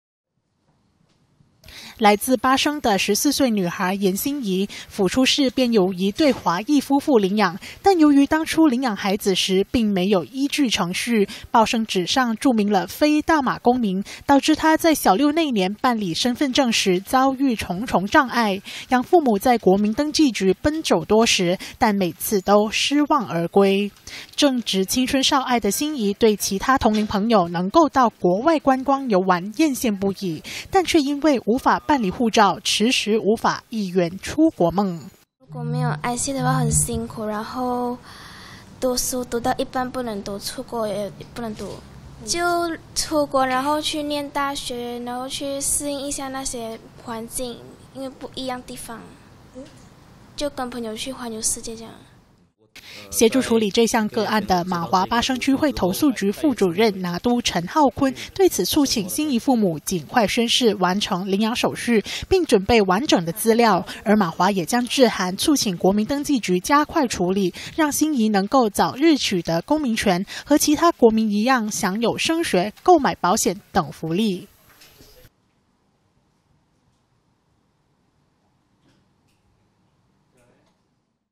领养手续没办妥无身份证 华裔少女盼获公民权 [20161123]
雪兰莪巴生一名在华裔家庭长大的14岁女孩颜欣宜，因为养父母当年走捷径、领养孩子，没有办妥领养手续，导致欣宜在12岁生日时，无法顺利申请大马卡，女孩一家在无计可施下，求助马华，希望能够尽快取得公民权，一圆出国升学梦。